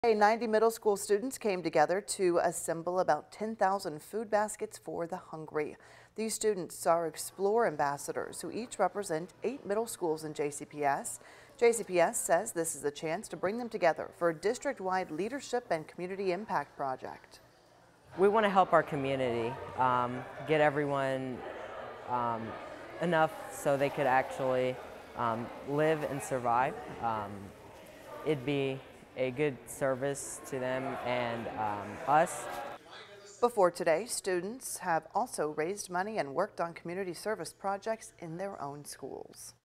Kentucky middle school students assemble 10,000 food baskets for the hungry
These students are explore ambassadors who each represent 8 middle schools in JCPS.